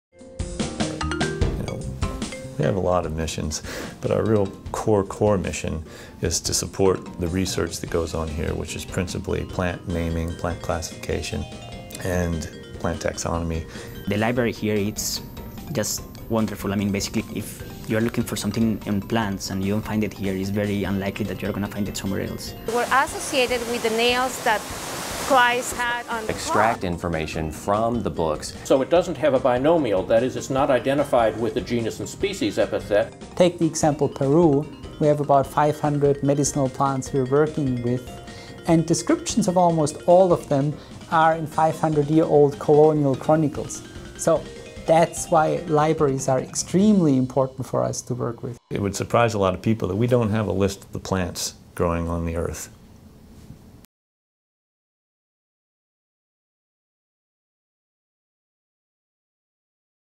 GLOW 102 - Missouri Botanical Garden and Herbarium
The Missouri Botanical Garden Library was founded in 1859 by St. Louis businessman Henry Shaw. It is one of the finest botanical libraries in the world, used locally—in conjunction with the herbarium—by horticulturists, students, landscapers and Garden members. Globally, it is used by historians, biographers and (most importantly) botanists, as part of their daunting mission: to identify and catalog every plant in the world.